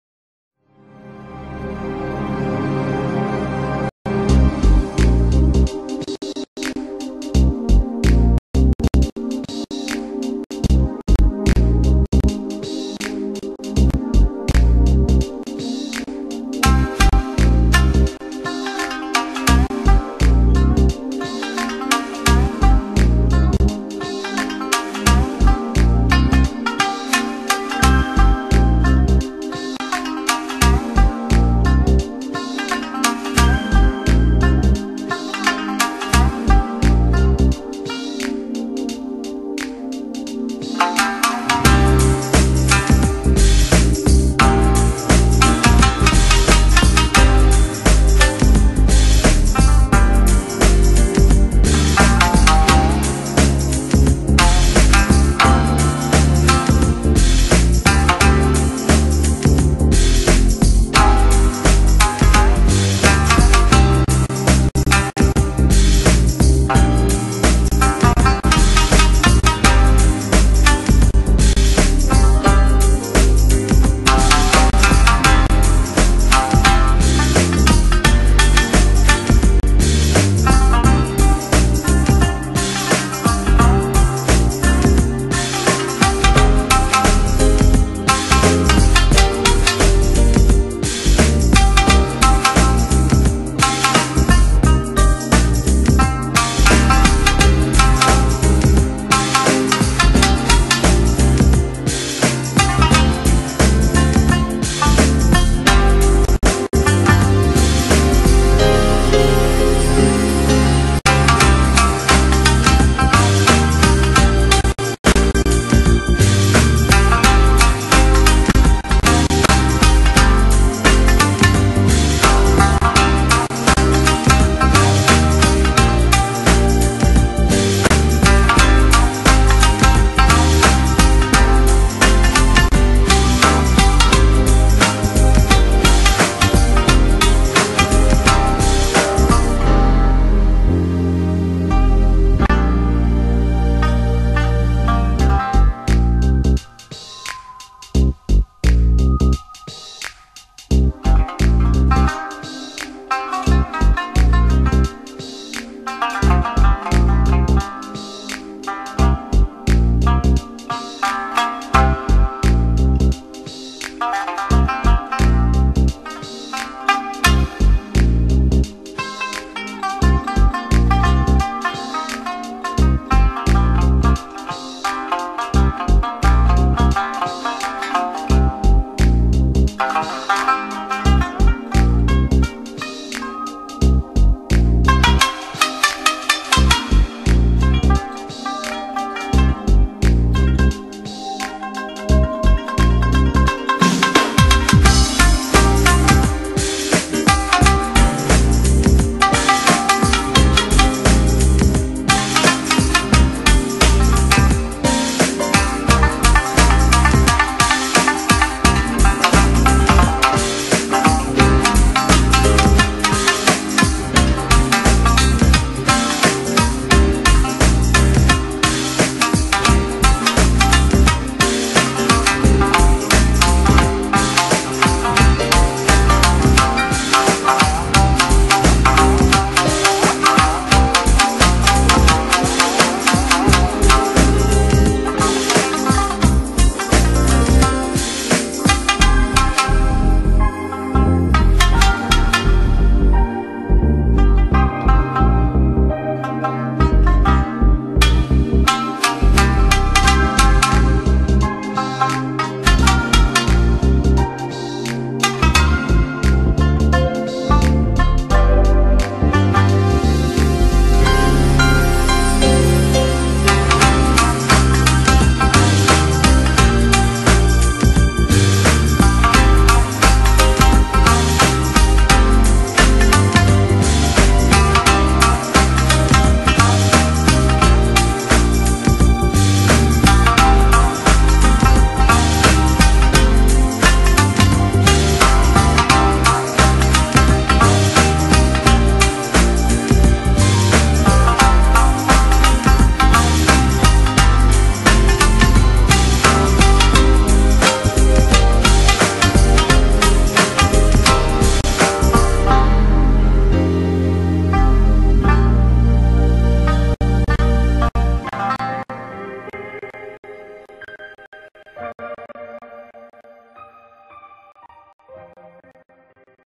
Japan Airlines, All Nippon Airways and JAS - A Tribute to Japanese Aviation
This is my first editied movie, a simple tribute to the well rounded commercial aviation industry in Japan.